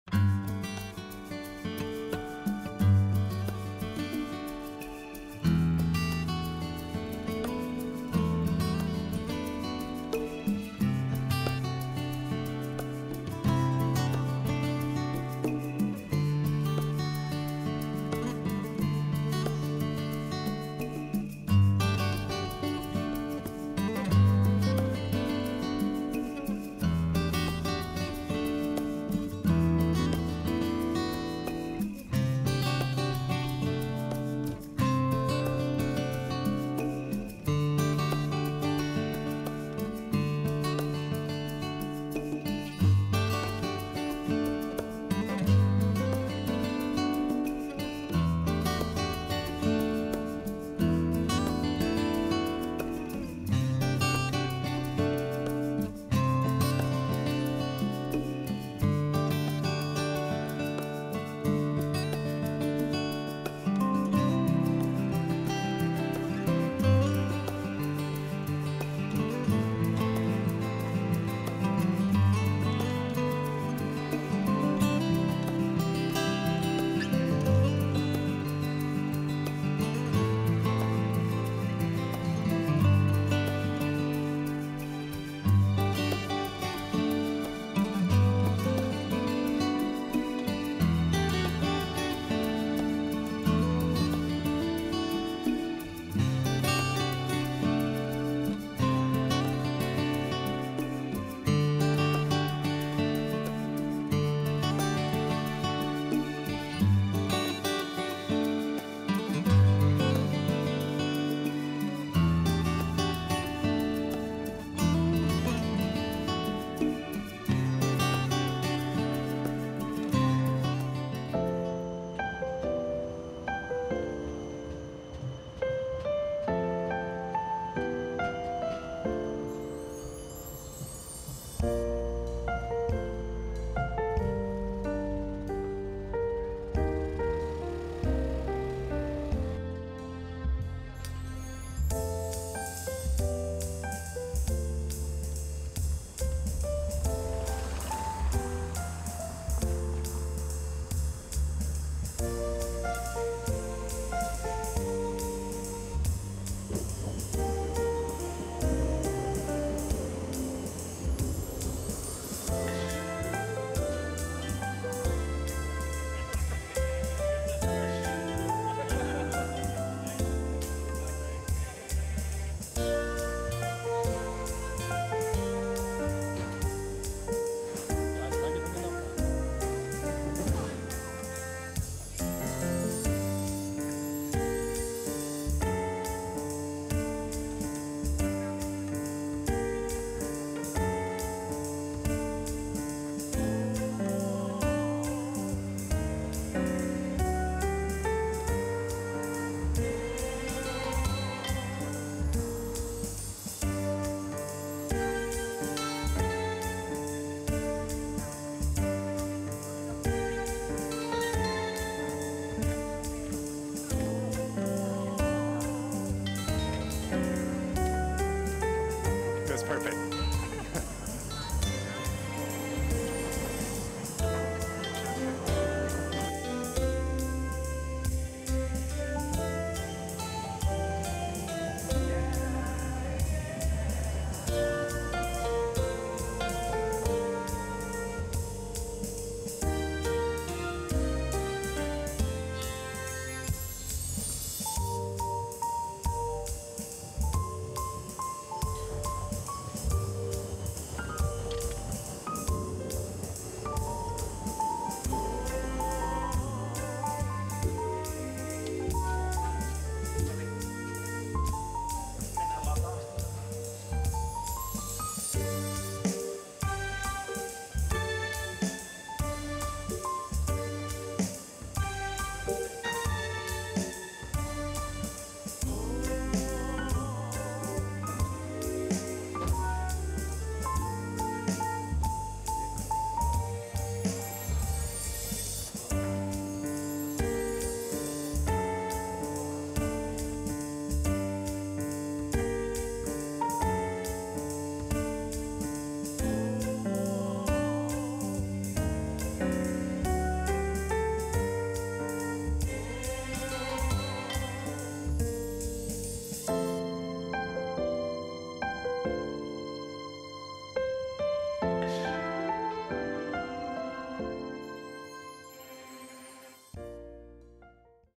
[멍하니] 스위스 시내를 보면서 5분간 멍때리기
해외여행 가고 싶은 이때 스위스의 아름다운 시내를 구경하세요